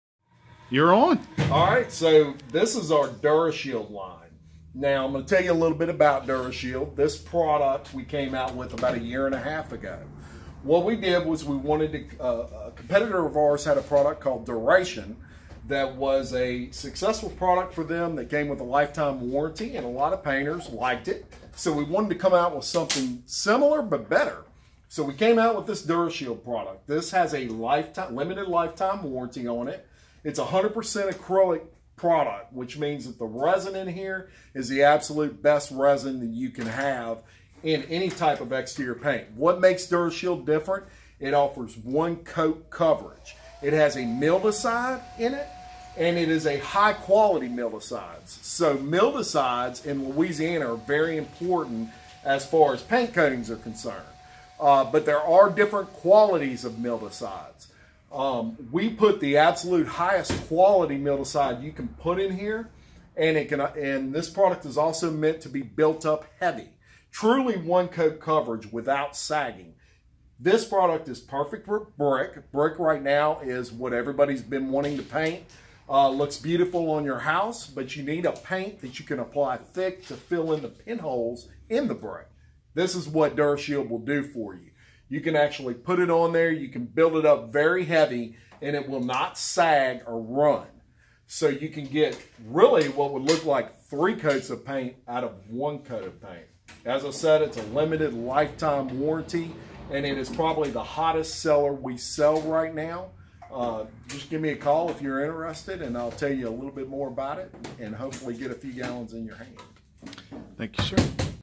Farrell-Calhoun's Jim Chapman discusses new paint line
Jim Chapman, Denham Springs franchise owner of Farrell-Calhoun, discusses his company's new paint line - Durashield. The paint comes with a limited lifetime warranty and offers a rare 'satin' finish for outdoor use.